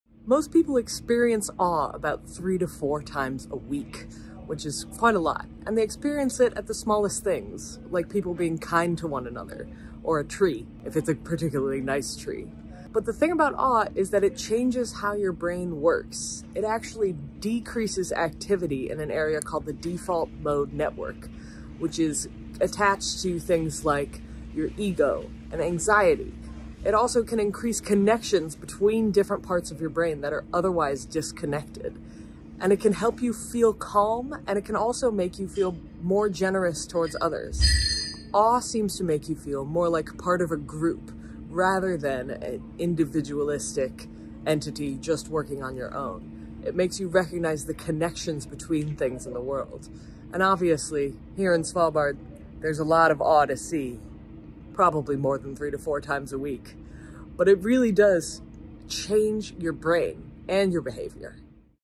The brain benefits of awe 😃
Most people experience awe about 3 to 4 times a week and can experience it at the smallest things, like people being kind to one another or a particularly nice tree. And the thing about awe is that it changes how your brain works, decreasing activity in an area called the default mode network, which is attached to things like your ego and anxiety.  It really does change your brain and your behaviour for the better.